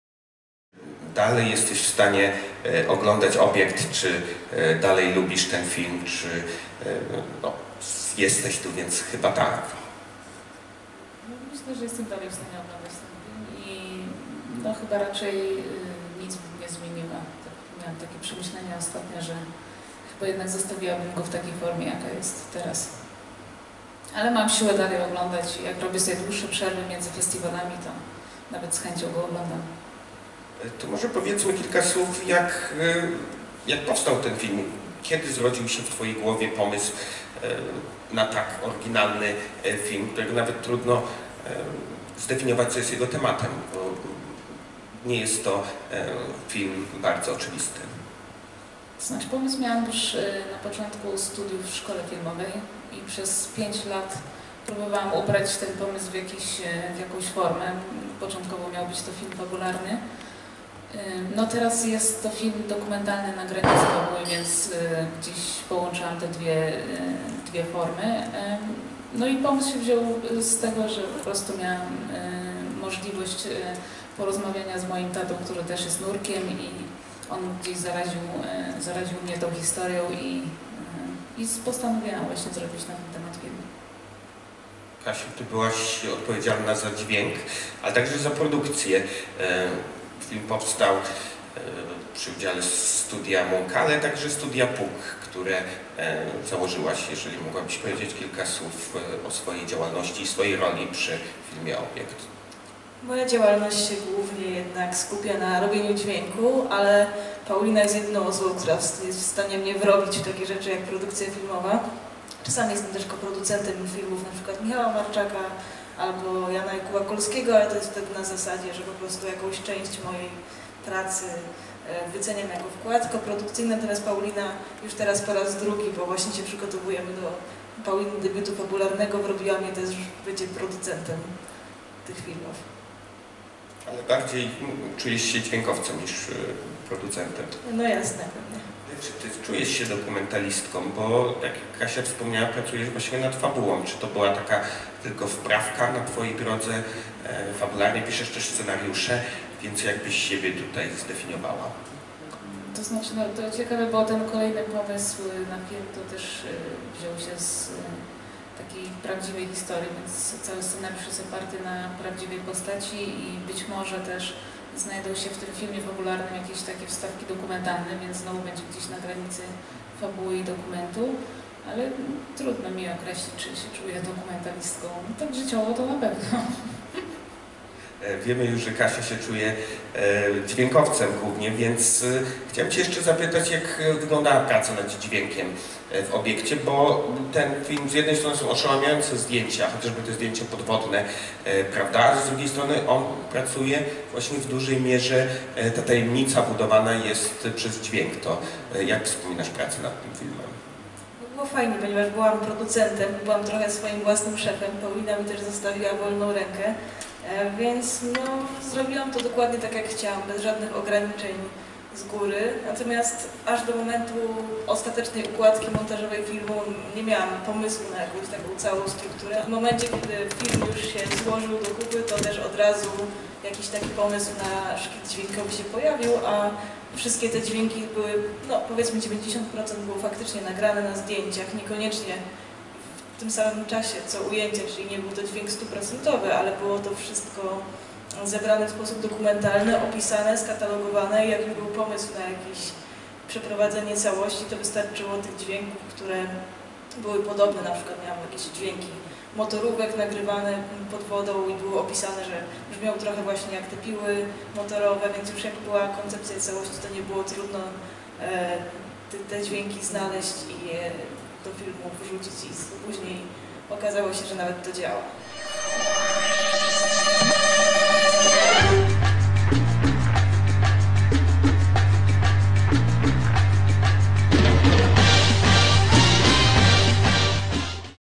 OFF CINEMA 2016: Paulina Skibińska i Katarzyna Szczerba - "Obiekt"
Reżyserka Paulina Skibińska i producentka Katarzyna Szczerba o swoim "Obiekcie" na 20 OFF CINEMA. Nie mają go jeszcze dosyć? Jeździ od festiwalu do festiwalu od zeszłego roku.